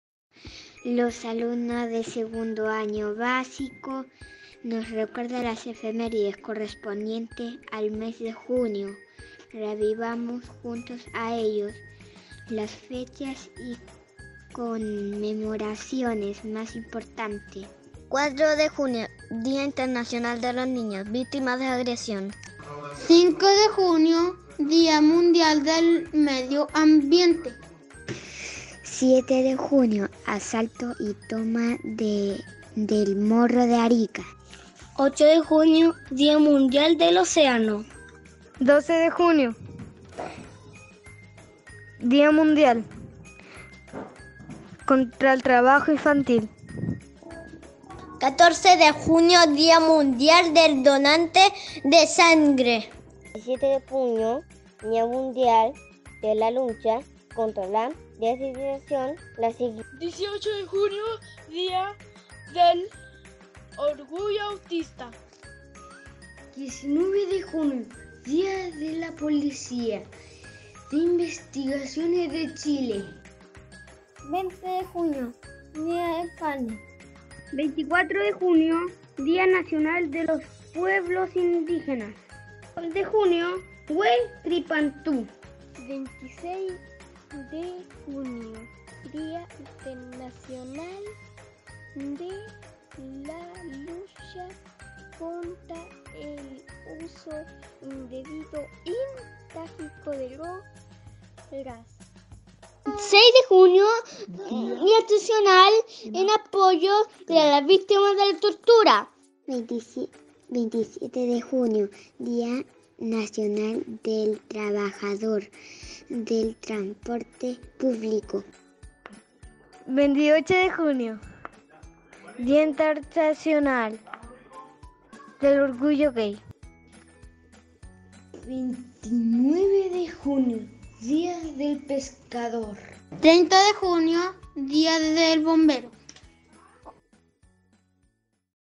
Efemérides Junio 2021, 2º año básico, Profesoras Ana Becerra Ahumada y Pia Enero Palma.
Efemérides Junio 2021, 2º año básico, Profesoras Ana Becerra Ahumada y Pia Enero Palma. Escuela Quecheregua de Pichingal. Junio 2021